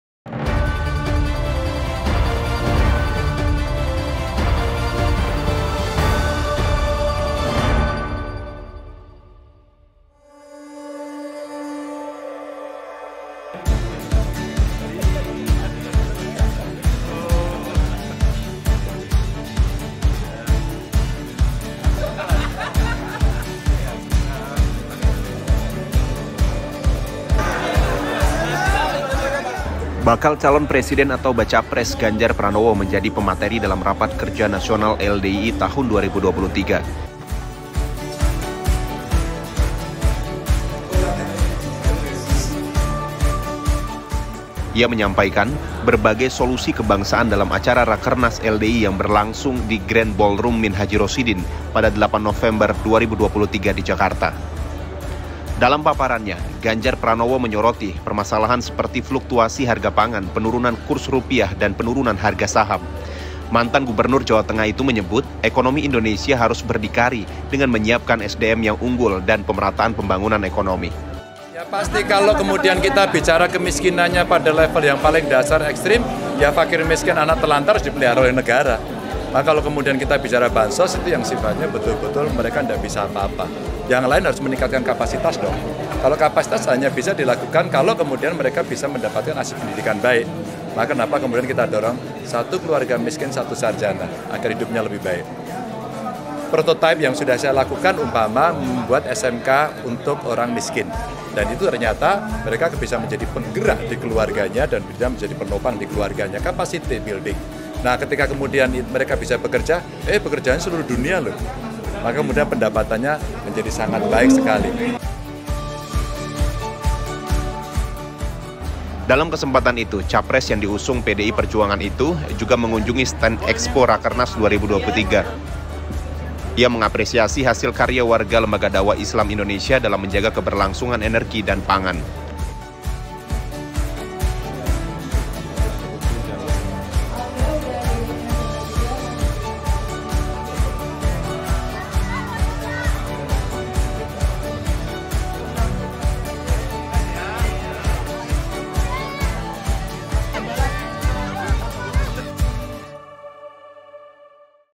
LDII TV: Rakernas LDII, Tokoh Nasional Ganjar Pranowo Ungkap Strategi Menuju Indonesia Unggul
Jakarta (8/11). Indonesia mengalami berbagai permasalahan seperti fluktuasi harga pangan, penurunan kurs rupiah dan penurunan harga saham. Untuk mengatasinya, bakal calon presiden (Bacapres) Ganjar Pranowo menjabarkan strategi “Menuju Indonesia Unggul”.

“Ekonomi Indonesia harus berdiri di atas kaki sendiri. SDM harus siap, dan diperlukan pemerataan pembangunan ekonomi,” ujar Ganjar saat memberikan pembekalan “Konsep dan Strategi Mewujudkan Indonesia Emas 2045”, di Rakernas LDII 2023, di Grand Ballroom Gedung Serbag Guna (GSG) Pondok Pesantren (Ponpes) Minhaajurrosyidin, Jakarta, pada Rabu (8/11).

Facebook : Lembaga Dakwah Islam Indonesia